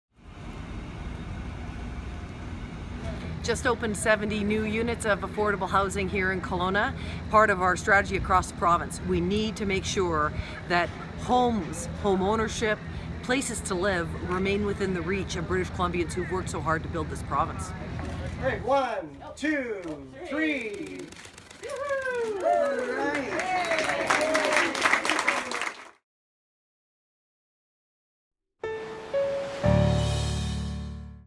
Families and seniors benefit from new affordable homes in Kelowna
Families and seniors in Kelowna have a new, safe place to call home at the newly redeveloped Pleasantvale, which celebrated its official opening today.

The project features 50 affordable housing units for seniors and 20 affordable townhomes for families with low to moderate incomes, providing more people in the community with quality, affordable housing.